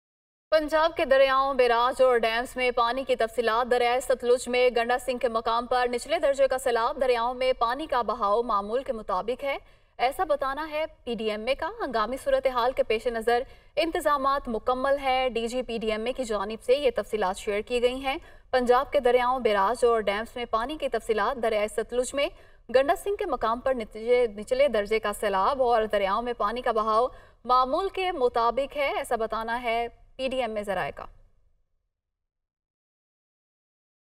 PDMA's Startling Details on Punjab's Rivers and Dams! Breaking News
In this video, we bring you the latest updates on the water flow in Punjab rivers and dams, as released by the Provincial Disaster Management Authority (PDMA).

The PDMA plays a crucial role in monitoring and assessing the situation of water flow in Punjab's rivers and dams. Their periodic releases of detailed information help keep the public informed about the current state of water resources in the region.

In this video, we will discuss the key details provided by the PDMA, including the water levels, flow rates, and conditions of rivers and dams across Punjab. We will delve into the significance of this information and how it impacts various aspects such as irrigation, hydroelectric power generation, and overall water resource management.

Join us as we analyze the implications of the latest situations of water flow in Punjab rivers and dams. Our experts will provide insights into the potential effects on agriculture, water supply, and environmental concerns.